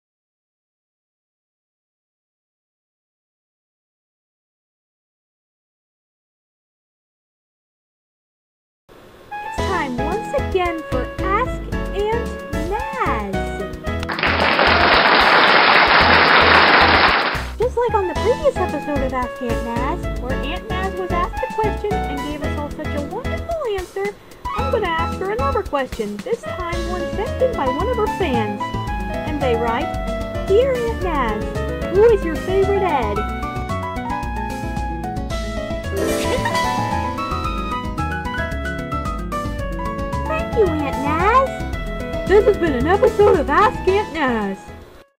Ask Aunt Nazz 2
To carry on the tradition of the "Ask Uncle/Aunt" series, I'm ending "Ask Aunt Nazz" with episode 2. I would've released it sooner, but NOBODY ASKED SOON ENOUGH!

Ahem, sorry. Recently, someone did, and here it is.